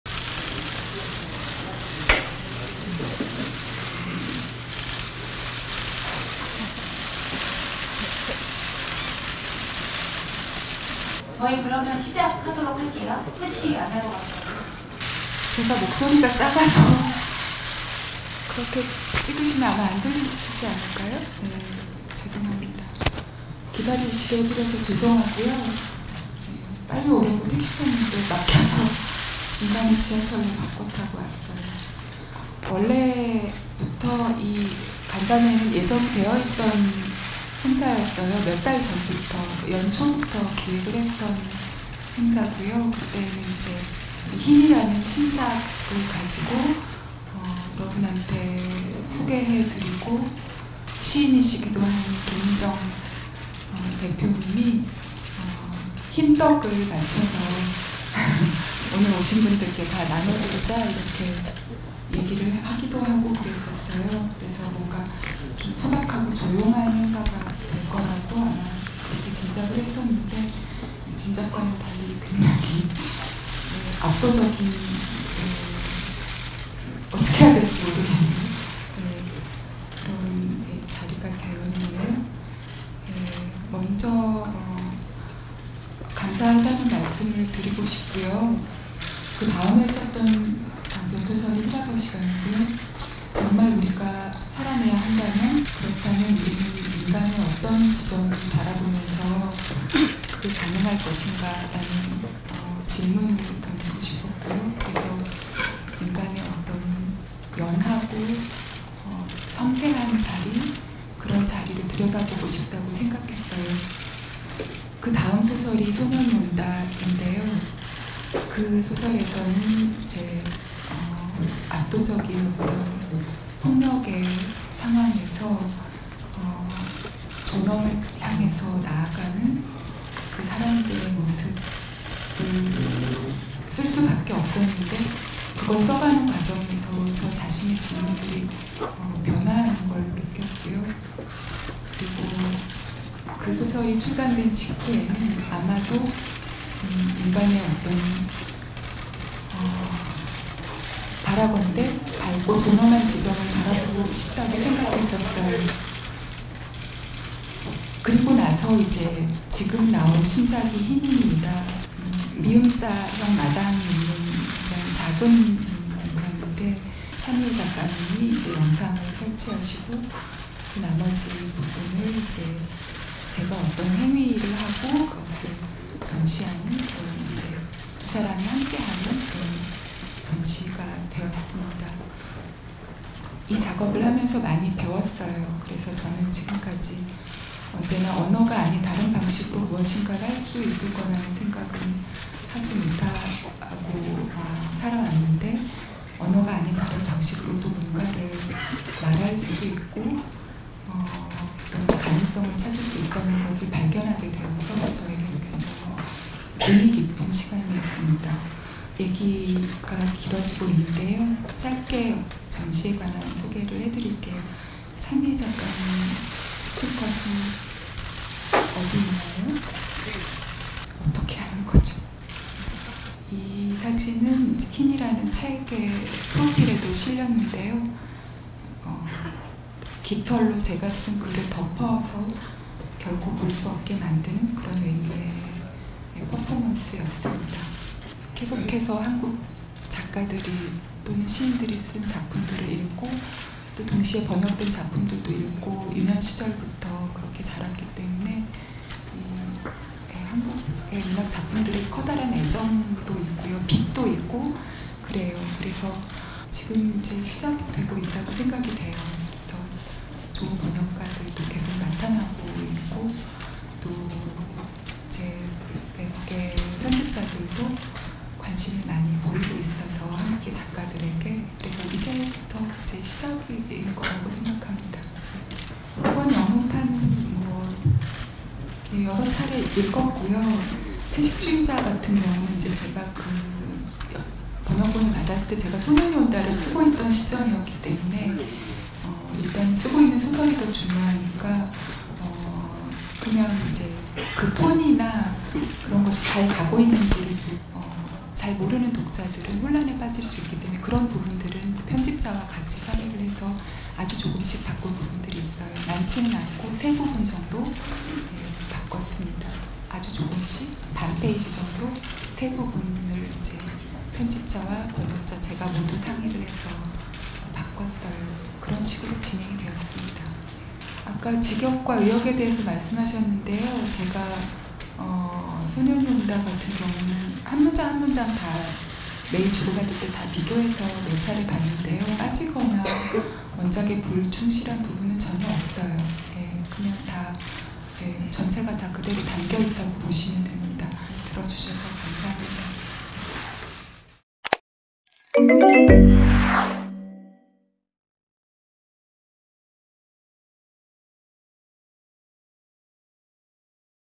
[2016 한강 기자회견 직캠] 플래시 세례에 당황한 한강, 물부터 들이켜더니..
한강 작가는 노벨문학상에 앞서 지난 2016년 한국인 최초로 세계적인 문학상인 ‘맨부커 인터내셔널’상을 받았는데요, 당시 예상치 못한 맨부커 수상으로 한국문학계의 스타로 떠오른 한강의 기자간담회에는 100명이 넘는 취재진이 참석 작가에게 뜨거운 관심을 보였습니다. 현장 영상과 인터뷰 보시겠습니다.